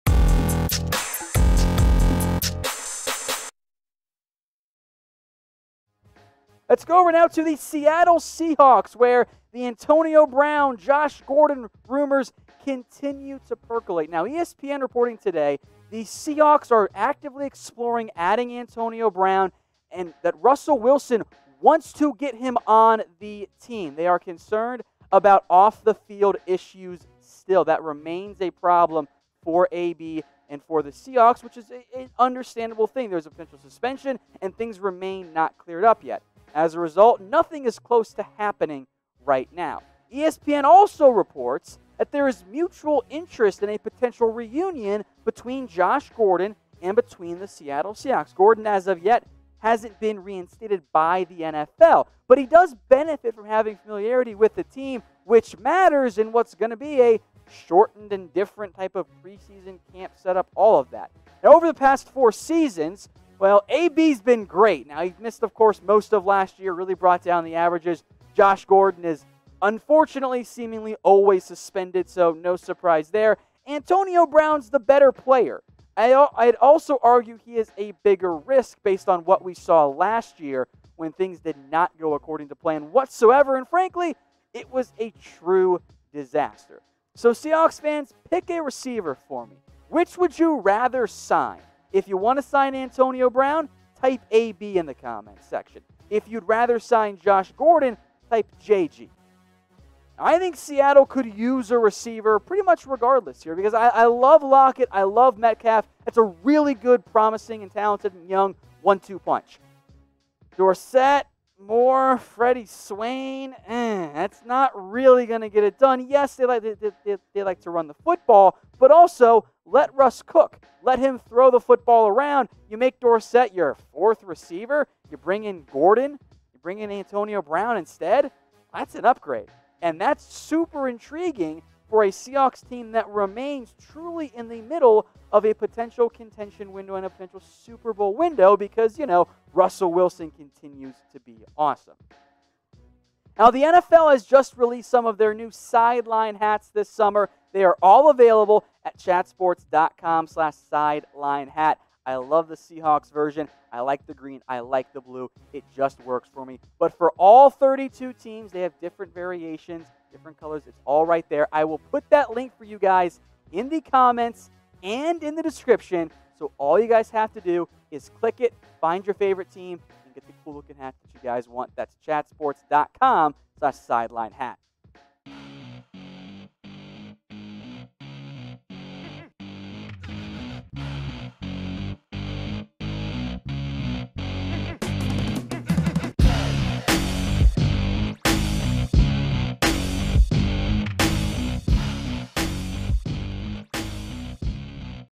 Seattle Seahawks Today: Update On Antonio Brown & Josh Gordon Rumors
Seattle Seahawks Today: ESPN brings fresh Seahawks rumors to us on Antonio Brown & Josh Gordon? PICK ONE TO SIGN - put it in the COMMENTS now!

Most Seahawks rumors this off-season have centered around Jadeveon Clowney, Antonio Brown and Josh Gordon. If Seattle makes another move before the 2020 NFL season, look for it to be for one of these guys.

12's! Click the big red button, and be on the lookout for more Seahawks videos right here from Chat Sports

Do Pete Carroll and John Schneider want to deal with the drama and off-field issues that come with AB or Josh Gordon? Yes, the Seahawks would love to get Russell Wilson another weapon next to Tyler Lockett and DK Metcalf in that WR corps, but are Brown or Gordon the right move with suspension or reinstatement outstanding issues, respectively?